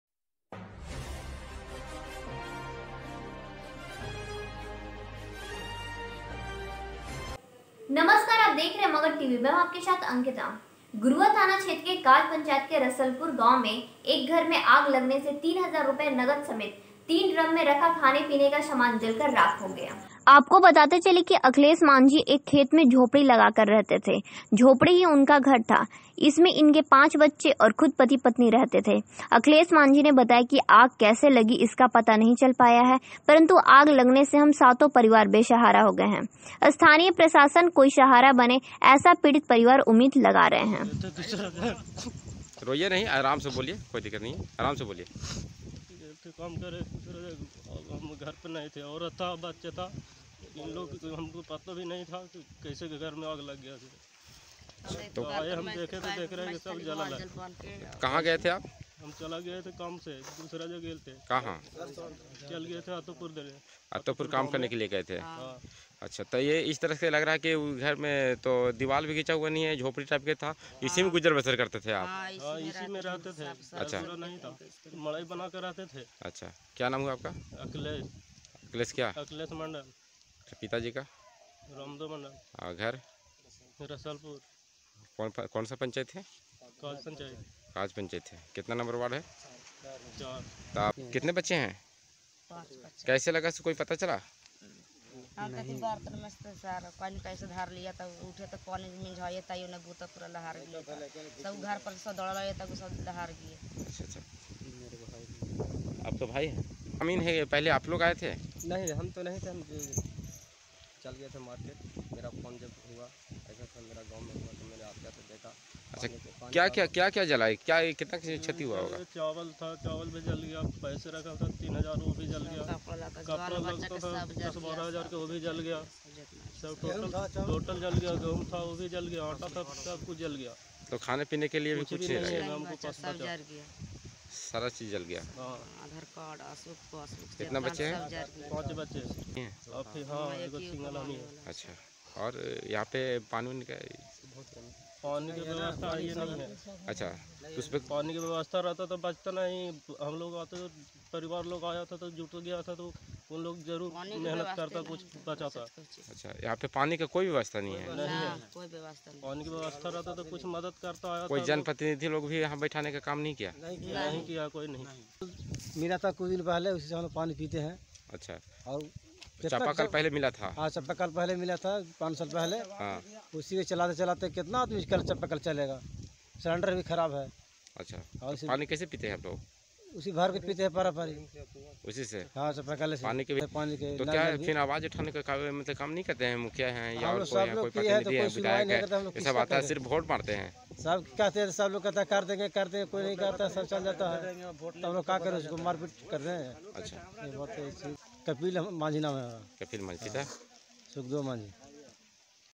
3000रुपया नगद और कई हजार रुपया का समान, अनाज जलकर राख और घर से बेघर हो गए मांझी परिवार
काज पंचायत के रसलपुर गांव में अखिलेश मांझी के घर में आग लगने से3000रुपया नगद और कई हजार रुपया का समान अनाज जलकर राख और घर से बेघर हो गए मांझी परिवार
 गुरुआ थाना क्षेत्र के काज  पंचायत के रसलपुर गांव में अखिलेश मांझी के घर में आग लगने से ₹3000 नगद समेत 3 ड्रम में रखा खाने पीने का सामान जैसे गेहूं और चावल और भी घर में रखे कपड़ा सारे समान जलकर राख हो गया बताते चलें कि अखिलेश मांझी एक खेत में झोपडी  लगाकर रहते थे झोपडी ही उनका घर था इसमें इनका 5 बच्चे और दो पति पत्नी रहते थे अखिलेश मांझी ने बताया की आग कैसे लगी इसका पता नहीं चल पाया है परंतु आग लगने से हम सातों परिवार  बेसहारा हो गए हैं स्थानीय प्रशासन कोई सहारा बने ऐसे पीड़ित परिवार उम्मीद लगा रहे हैं